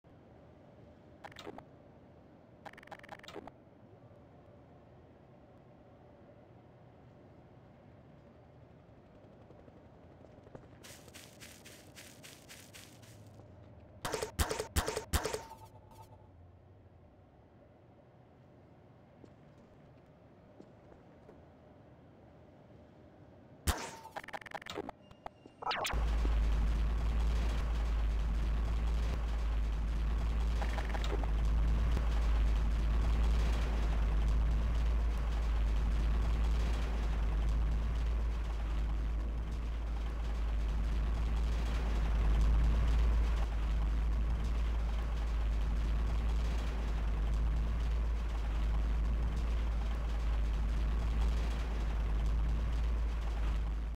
PAC3+Wire MODE(E2)
This is an airplane made in pac3+wiremod(flying base) And if it's simpler, the chip under the name "B.A.T" on the chair creates a flying base and the Body was made in pac3